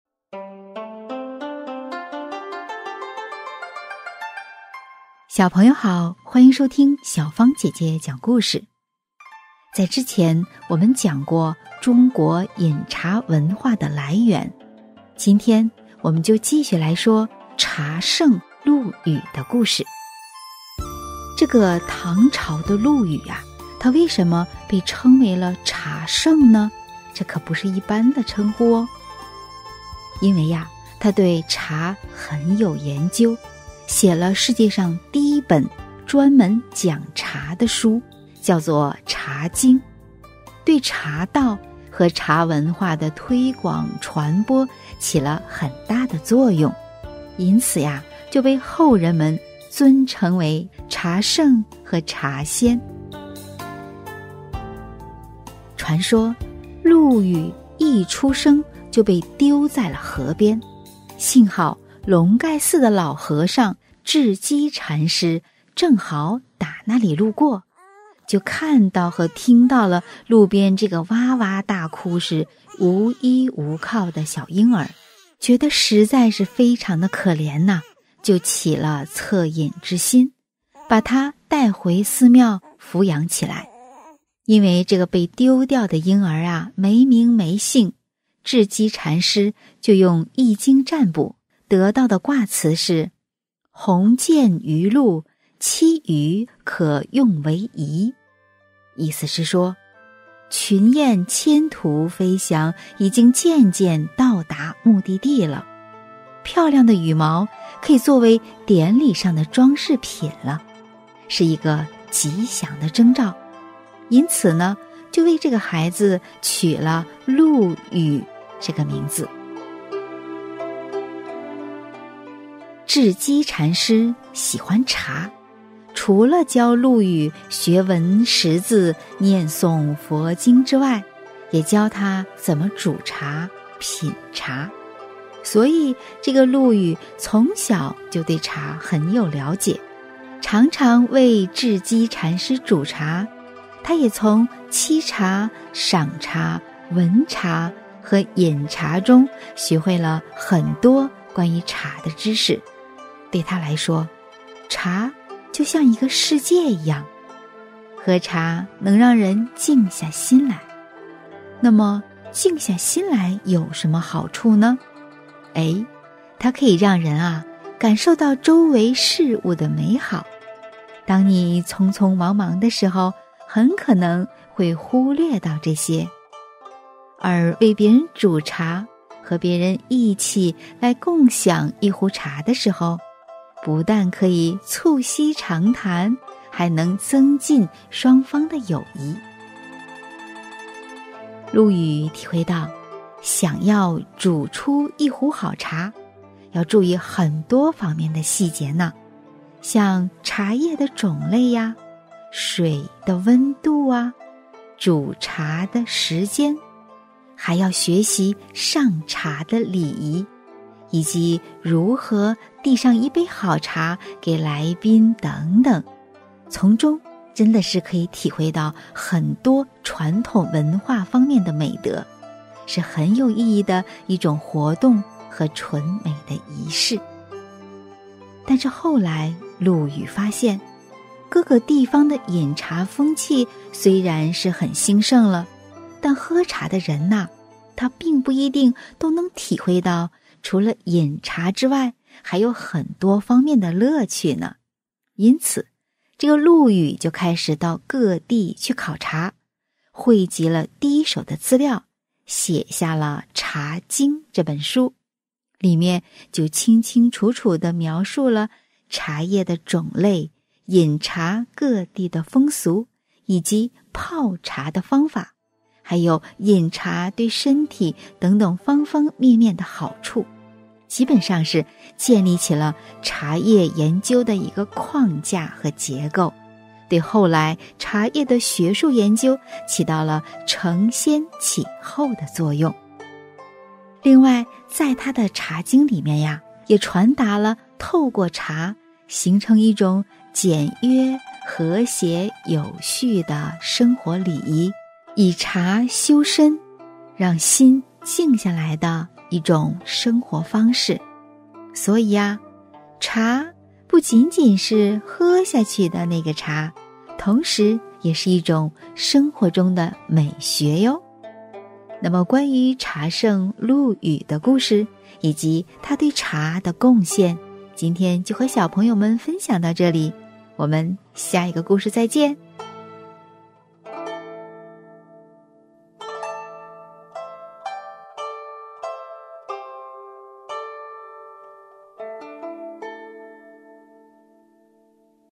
茶聖陸羽的故事（曉芳姐姐講故事/中華傳統文化）
小朋友們，我们已經知道俩中國飲茶文化的來源，那么今天来聊聊茶聖陸羽的故事！
這個唐朝的陸羽為什麼被稱為茶聖呢？因為他對茶很有研究，寫了世界上第一本專門講茶的書:《茶經》，對茶道和茶文化的推廣傳播起了很大的作用，因此被後人們尊稱為「茶聖」。